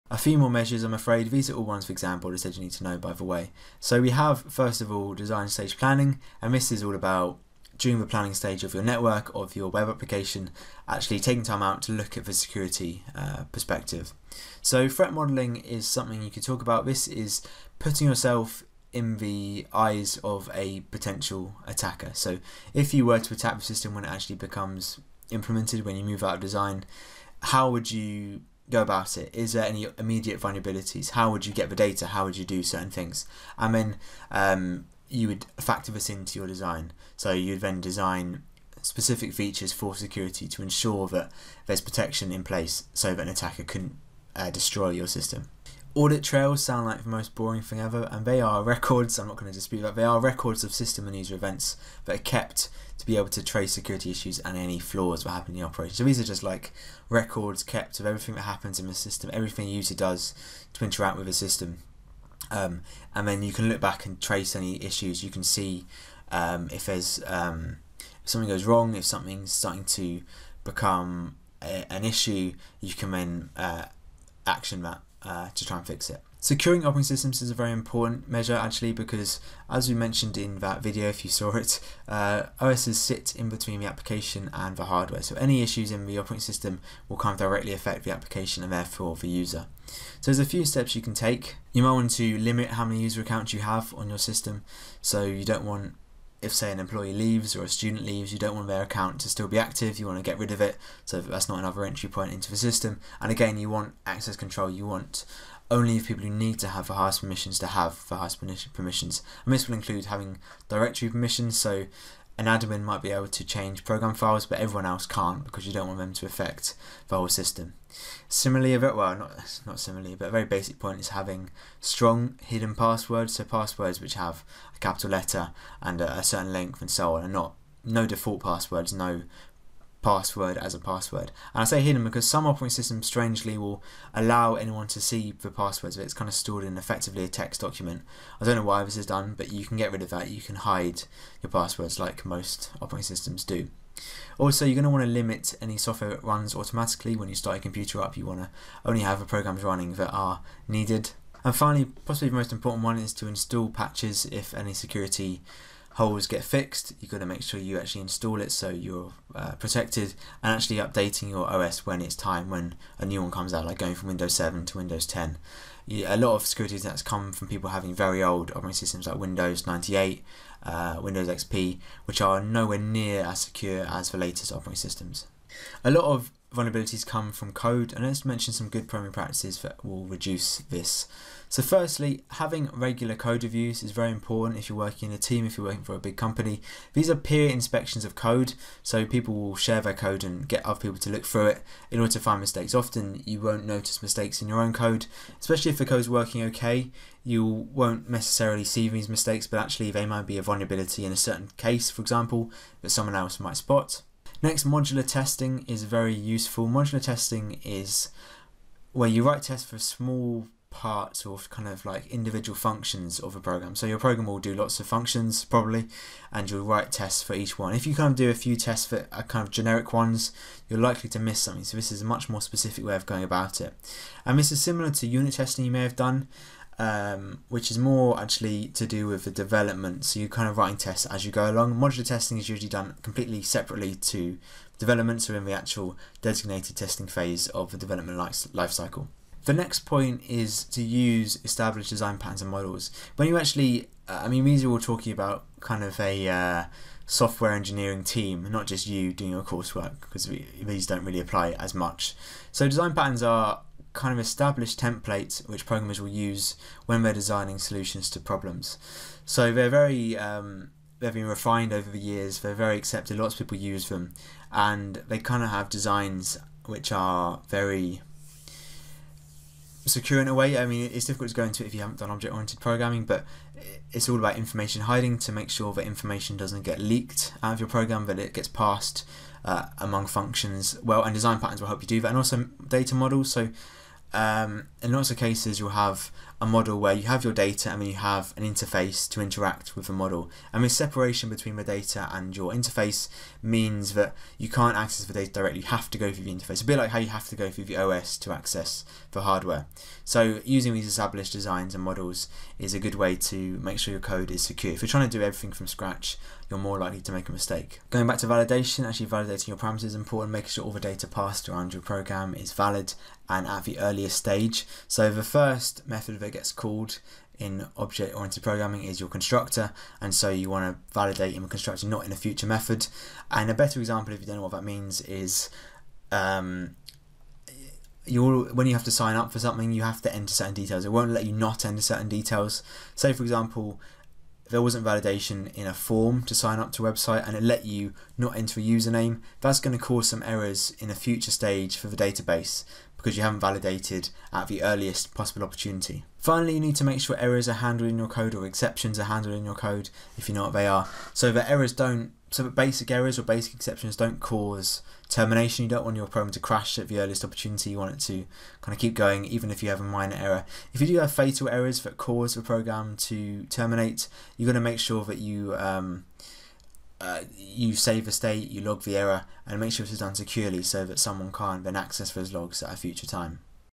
Network Security #3: More Security Measures
The final video on this topic - not a lot of fun I'm afraid, but Edexcel have specifically mentioned these security measures so they need to be covered!

These videos will always be free but if you'd consider a donation I'd be extremely grateful